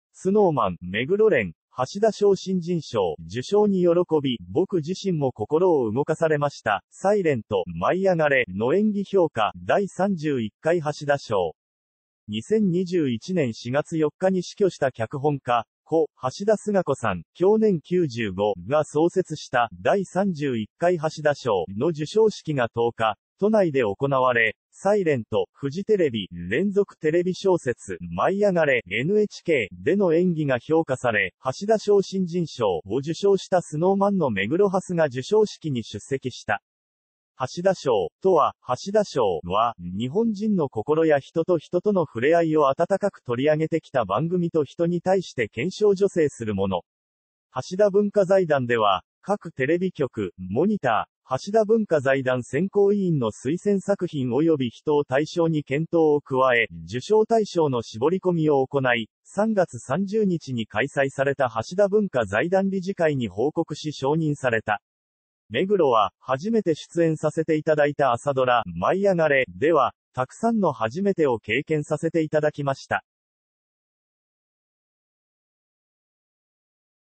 Snow Man・目黒蓮、「橋田賞新人賞」受賞に喜び「僕自身も心を動かされました」『silent』『舞いあがれ!』の演技評価〈第31回橋田賞〉
2021年4月4日に死去した脚本家、故・橋田壽賀子さん(享年95)が創設した「第31回橋田賞」の授賞式が10日、都内で行われ、「silent」(フジテレビ)、連続テレビ小説「舞いあがれ!」(NHK)での演技が評価され「橋田賞新人賞」を受賞したSnow Manの目黒蓮が授賞式に出席した。
■ 「橋田賞」とは 「橋田賞」は、日本人の心や人と人とのふれあいを温かくとりあげてきた番組と人に対して顕彰助成するもの。橋田文化財団では、各TV局、モニター、橋田文化財団選考委員の推薦作品及び人を対象に検討を加え、受賞対象の絞込みを行い、3月30日に開催された橋田文化財団理事会に報告し承認された。目黒は「初めて出演させていただいた朝ドラ『舞いあがれ！』では、たくさんの初めてを経験させていただきました。

IMPORTANT!